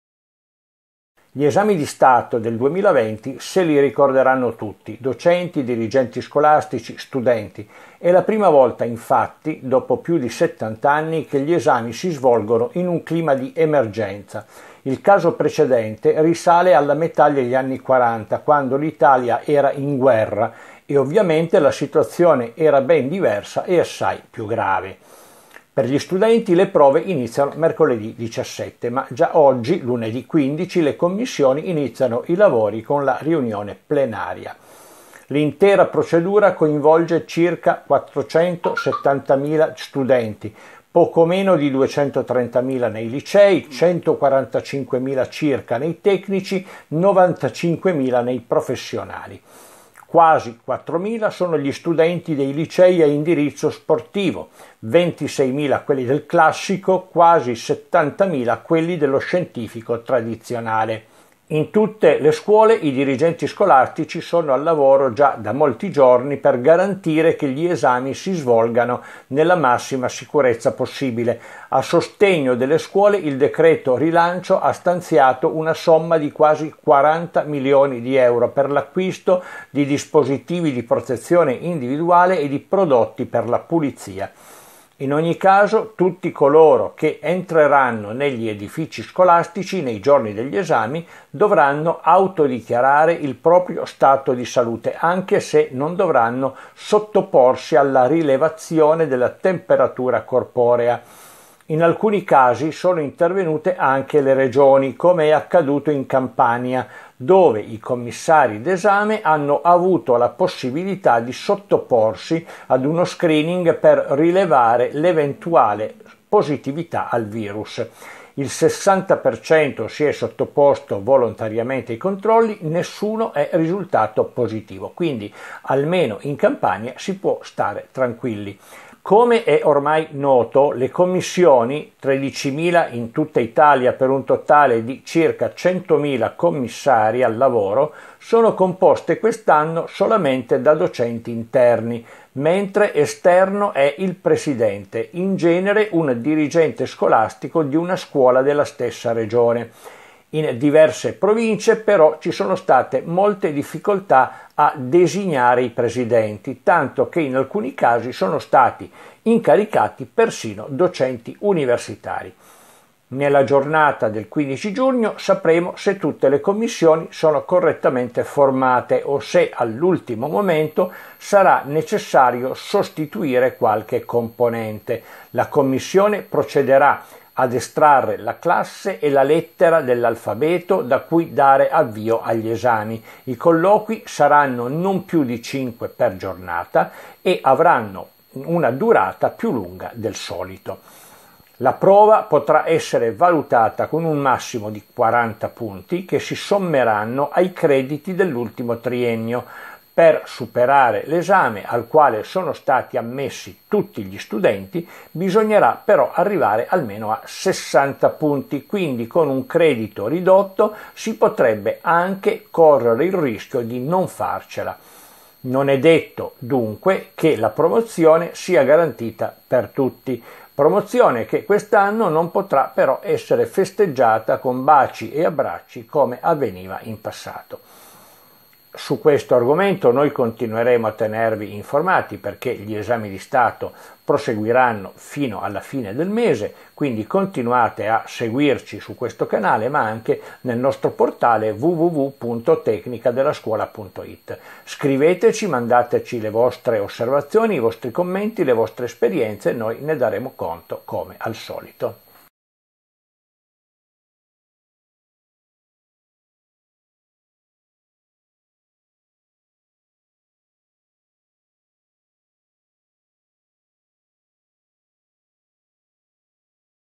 Esami di Stato per 470mila studenti. Impegnati 100mila commissari
– Quelli del 2020 sono gli esami di Stato dell’emergenza. Il caso precedente risale a più di 70 anni fa, ma all’epoca l’Italia era in guerra
– Il 15 giugno sono in programma le riunioni plenarie delle commissioni; le prove iniziano il giorno 17
– Questi i numeri: 470mila studenti, poco meno di 230mila nei licei, 145mila circa nei tecnici, 95mila nei professionali
– Per consentire lo svolgimento degli esami in sicurezza ci sono quasi 40 milioni per stanziati dal Decreto Rilancio
– Tutti coloro che entreranno negli edifici scolastici nei giorni degli esami dovranno autodichiarare il proprio stato di salute
– In Campania il 60% dei commissari d’esame si è sottoposto ad uno screening, nessuno di loro è risultato positivo
– Le commissioni (13mila in tutto) sono composte da docenti interni, il presidente è esterno
– In molti casi ci sono state difficoltà per reperire i presidenti; si vedrà il 15 giugno se tutto potrà procedere con regolarità
– La commissione procederà ad estrarre la classe e la lettera da cui dare avvio agli esami. I colloqui saranno non più di 5 per giornata
– La prova sarà valutata con un massimo di 40 punti che si sommeranno ai crediti dell’ultimo triennio
– Per superare l’esame lo studente deve arrivare ad almeno a 60 punti.
Con un credito ridotto si rischia di non farcela. Non c’è la promozione garantita.